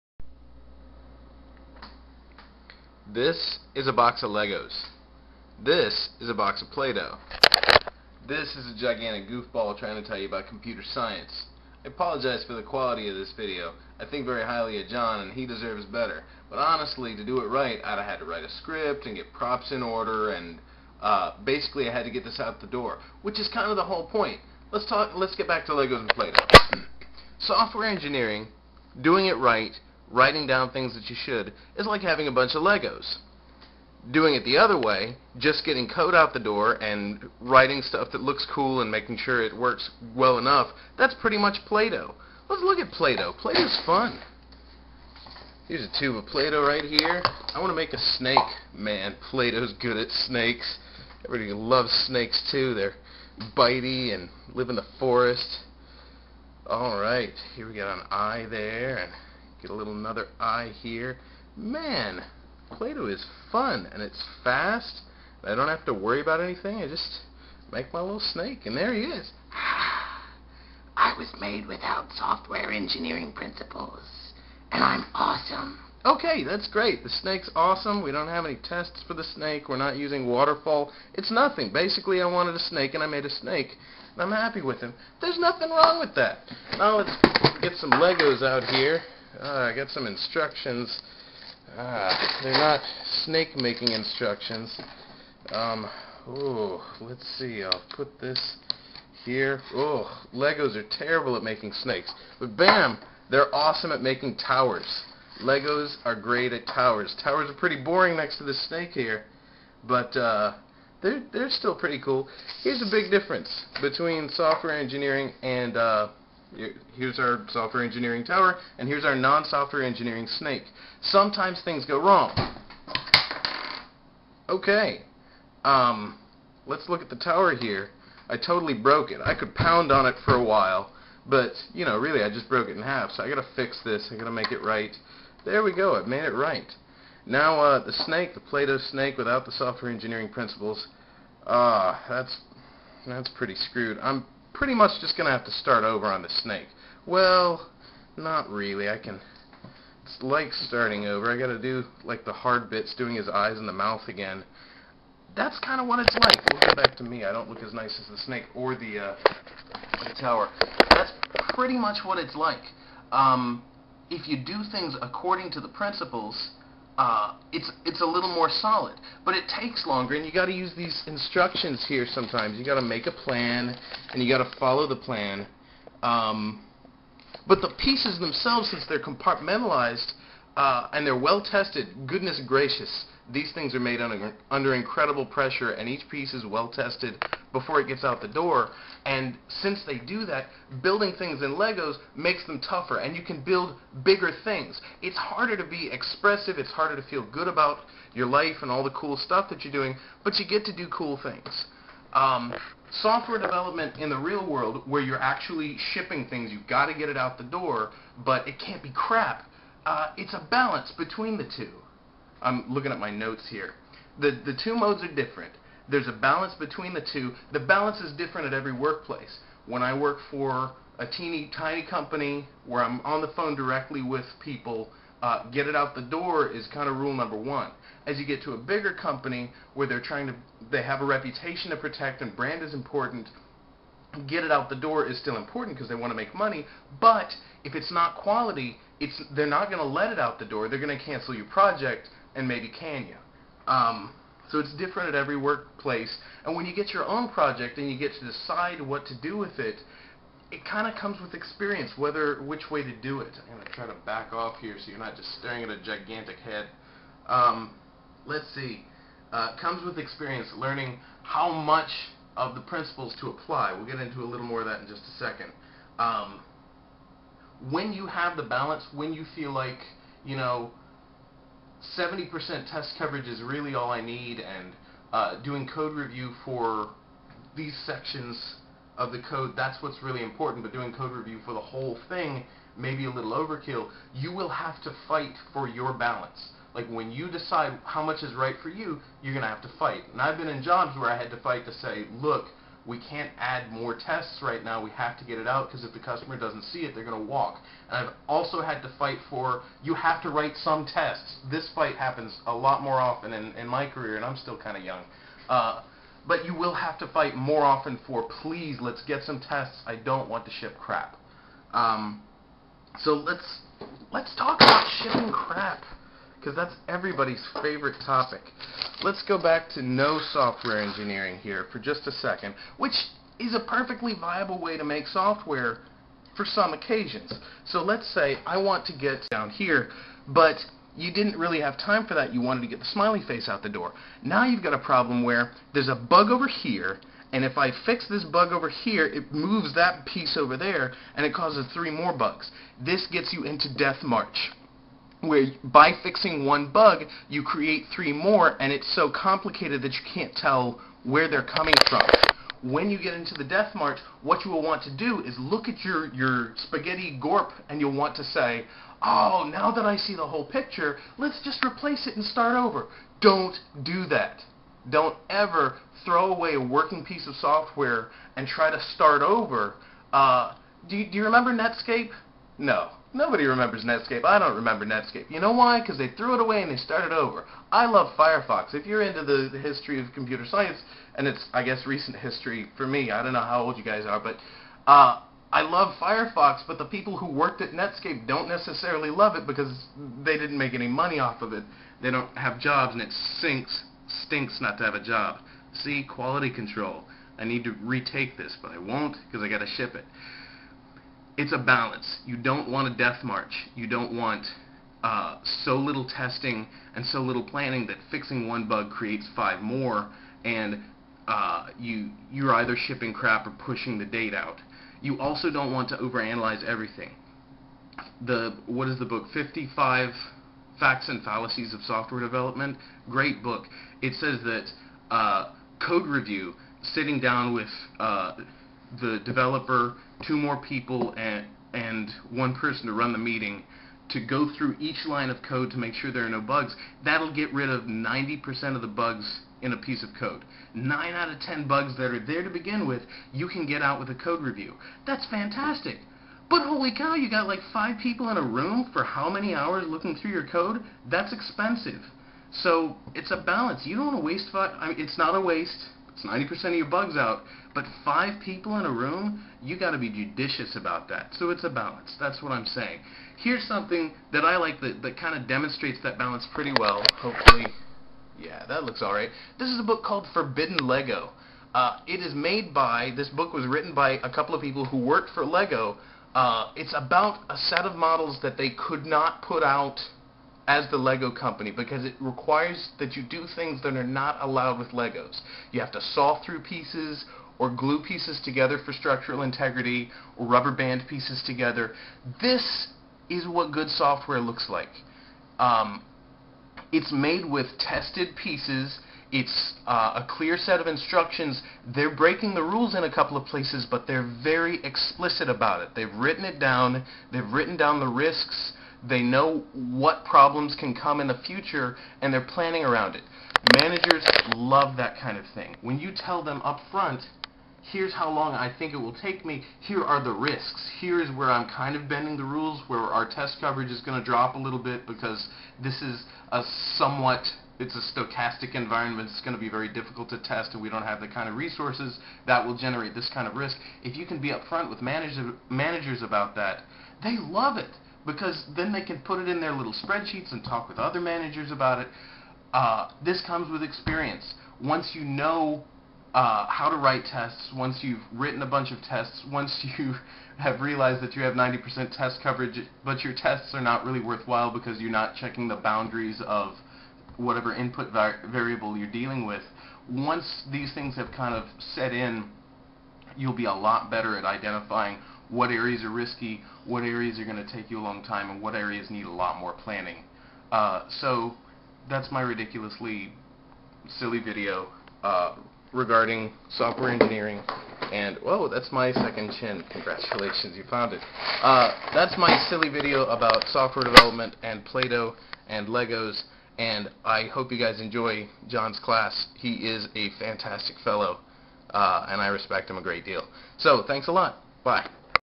Software Engineering with Legos and Play-Doh
I didn't plan ahead, so it was longer than 10 minutes and greater than 300 MB.  It ended up taking me way longer than I had intended.  Software Engineering practices save you time in the long run by forcing you to deal with the realities of complex processes.